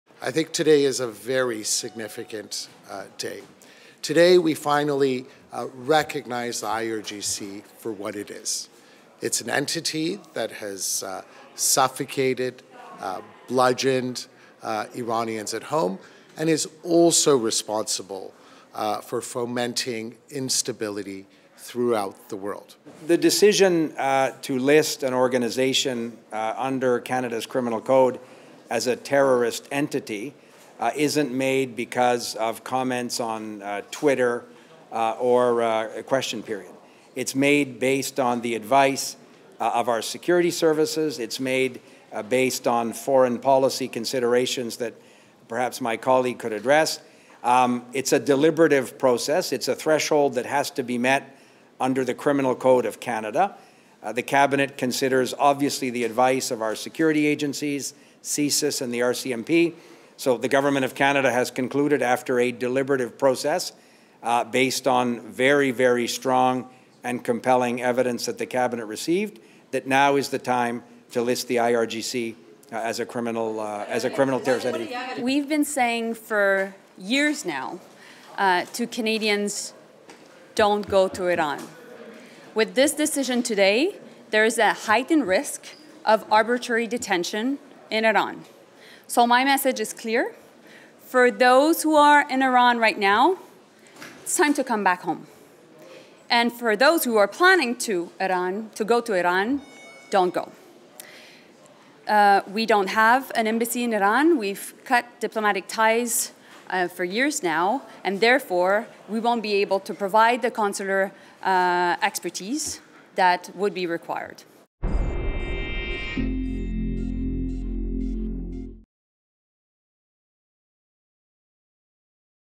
Canada lists Iran's Islamic Revolutionary Guard Corps as terror group
Public Safety Minister Dominic LeBlanc says Canada has listed Iran's Islamic Revolutionary Guard Corps as a terrorist entity, following months of mounting political pressure to do so. The Liberals have said in the past that listing the group could affect a number of people who had no choice but to be drafted into the organization. (June 19, 2024)